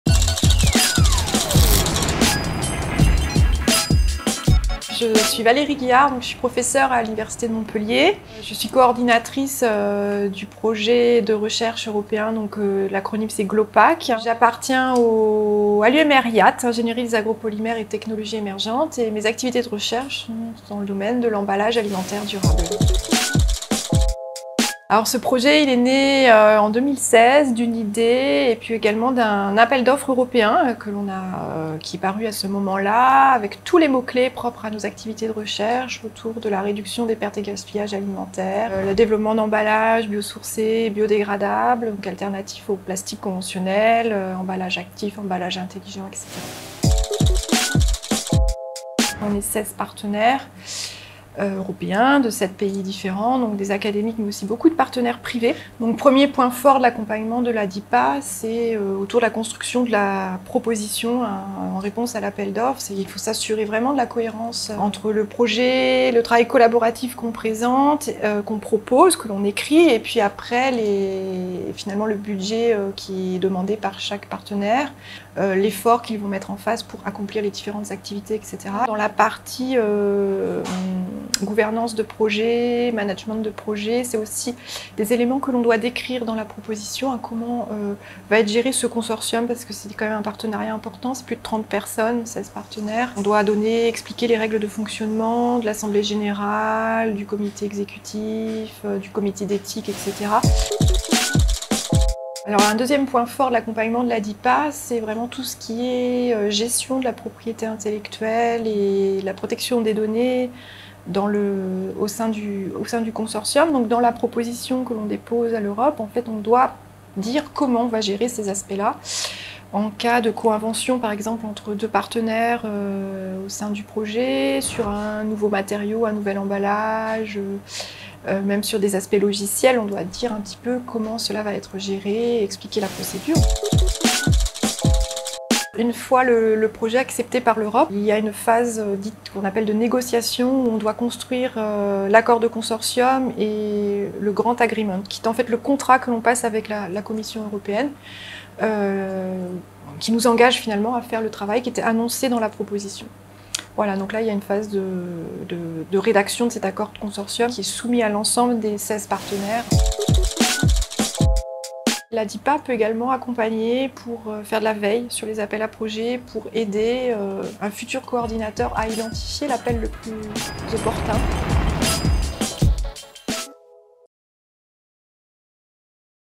Décollage #01 / Être lauréat d’un appel à projet européen (version EN)
Valérie Guillard, chercheuse au laboratoire IATE, est coordinatrice du projet de recherche Glopack dans le domaine de l’emballage alimentaire durable, lauréat d’un appel à projet européen Horizon 2020. Une belle réussite qu’elle nous présente ce mois-ci dans le cadre de la série vidéo « Décollage » proposée par la direction de l’innovation et des partenariats de l’Université de Montpellier (DIPA).